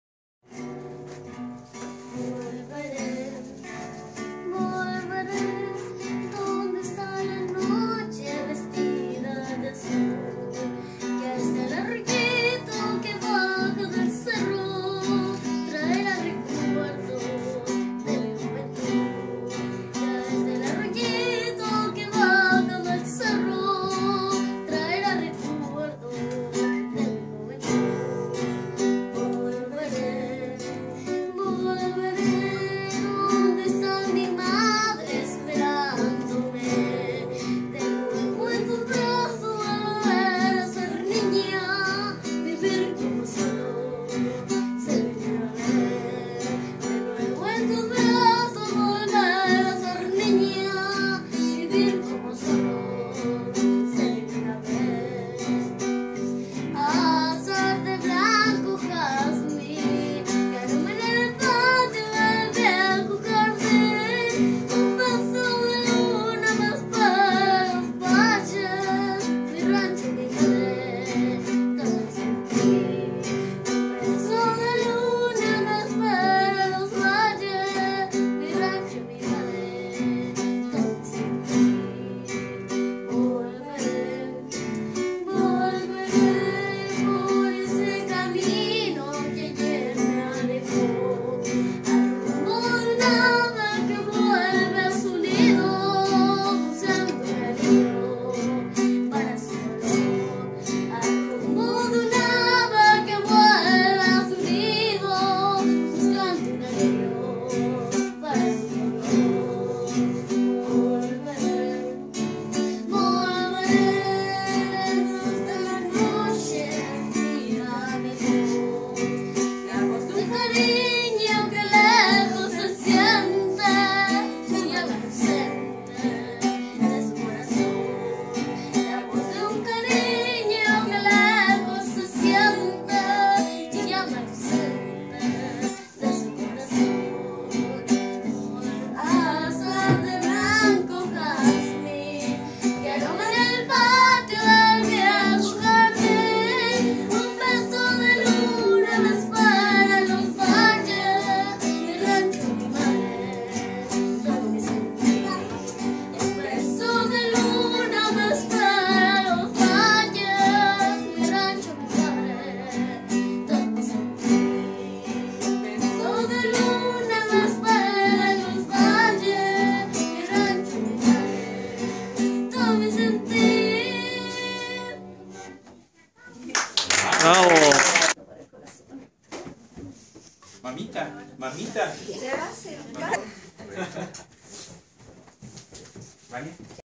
A las madres en la Comunidad de Huiro
La pequeña Sayén sorprende a todos con su talento musical, en especial a las madres en su día; homenaje que realizó al culminar la Liturgia de la Palabra que cada mes se vive en Huiro, gracias a la Visita Misionera de profesores, administrativos y estudiantes del Instituto Inmaculada Concepción Valdivia.